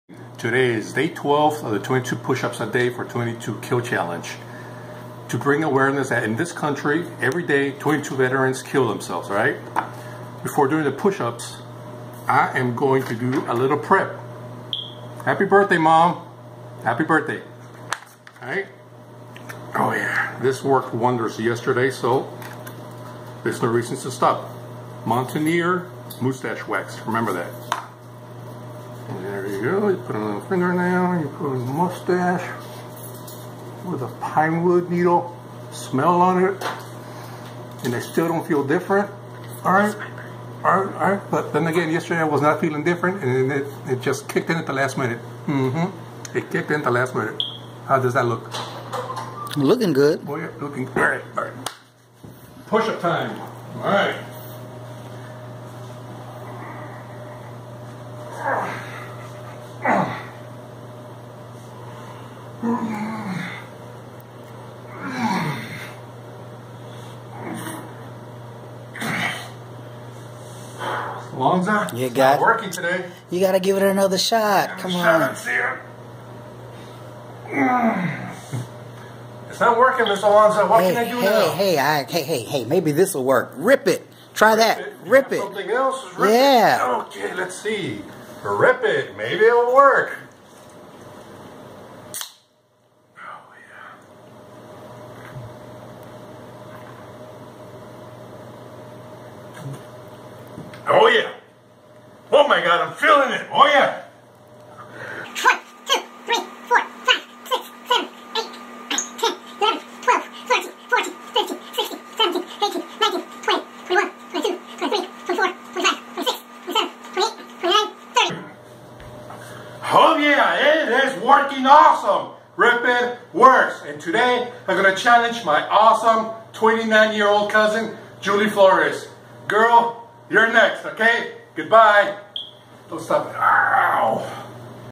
12 of 22 pushups challenge.
Today is day 12 of the #22 pushups for 22kill challenge. Rip It Energy rules‪!